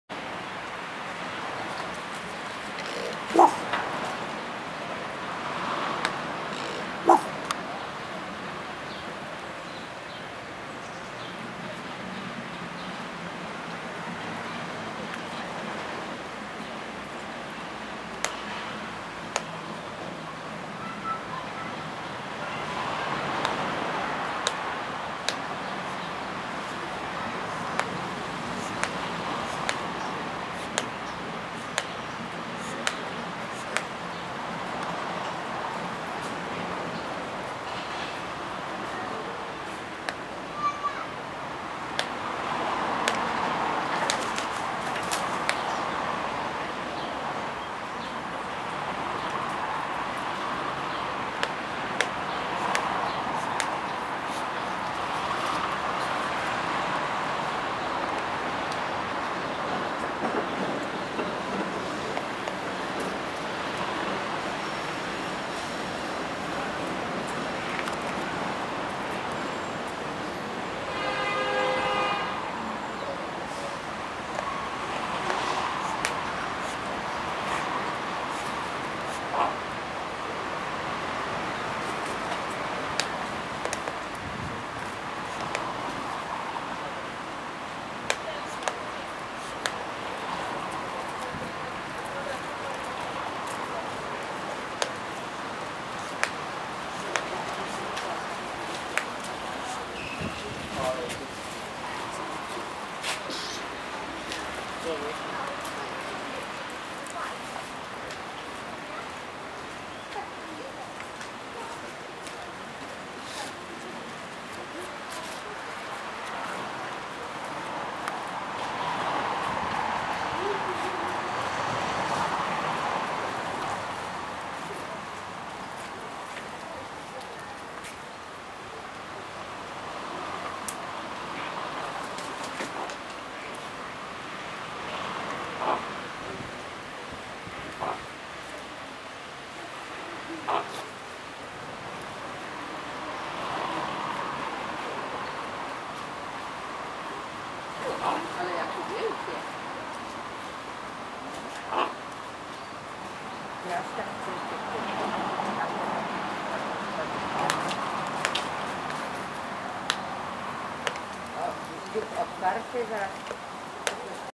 Common Raven in Wroclaw Zoo Kruk Corvus Corax
Dla odmiany zapraszam do relaksu we wrocławskim ogrodzie zoologicznym. Wsłuchując się w odgłosy otaczającej nas natury, można skutecznie zapomnieć o otaczającym nas świecie. Dlatego warto poświęcić chwilę uwagi.
This time, I invite you to watch the animals in Wroclaw Zoo, it is very nice and relaxing way to hang out. Observing surrounding nature will help to forget about all the unpleasant things that are currently bothering you. Enjoy.
This film was made with SONY HDR-XR160E